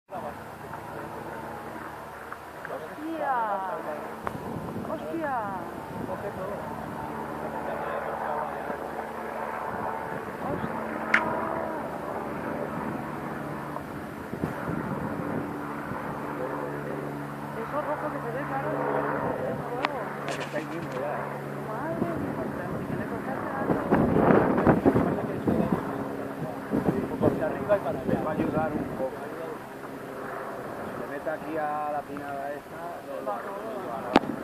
Incendio en el Parque Natural del Túria
Inendio en La Vallesa, Parque Natural del Túria, a su paso por Paterna y Riba-roja.